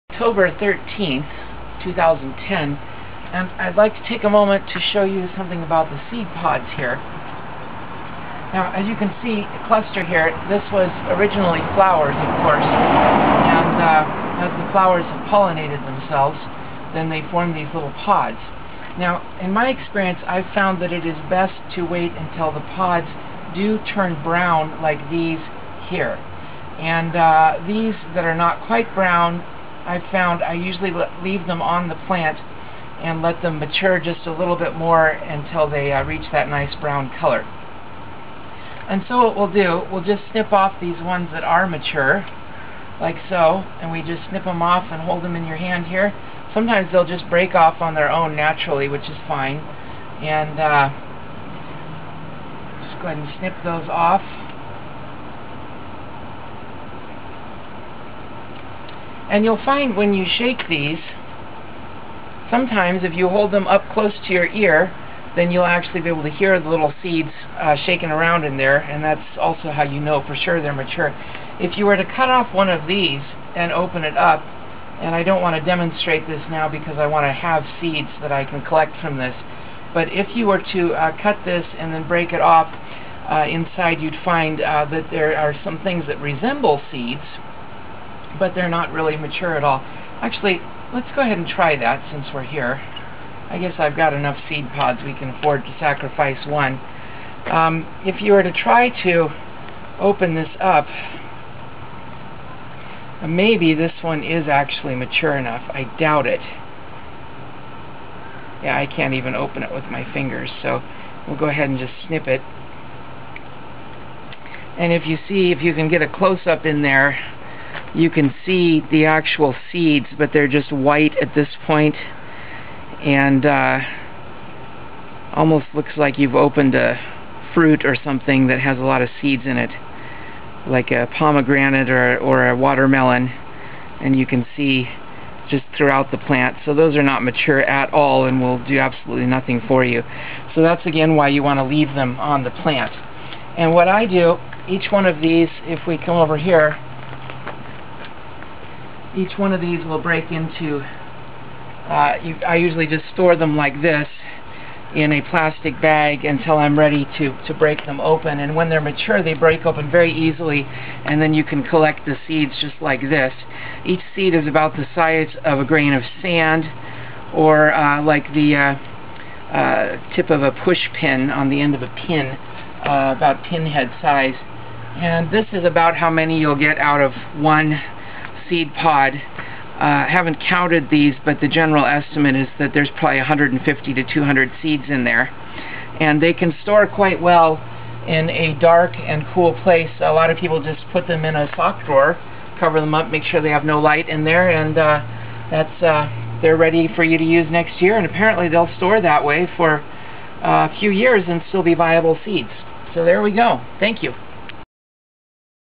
Harvesting seeds - how and when
(5522) Tobacco plants product BEAUTIFUL flowers, and after they're pollinated, they form seed pods. When the pods are fully brown (dry) on the plant, it's time for harvest. Each pod contains 75-150 seeds, sometimes 200, and are about the size of a grain of sand. or smaller. It's amazing how such a small seed can product such a big, BEAUTIFUL plant reaching 7 feet tall with 2-3 foot long leaves! Astounding! And one plant can produce up to 100 flowers, which can mature to many healthy clusters of individual pods. So even just ONE plant is capable of producing thousands of seeds.

This video discusses how the seed pods are harvested, and methods for storing them.  Also shown is an example of what can happen when a seed pod is plucked too soon.

Enjoy, and happy gardening!     :)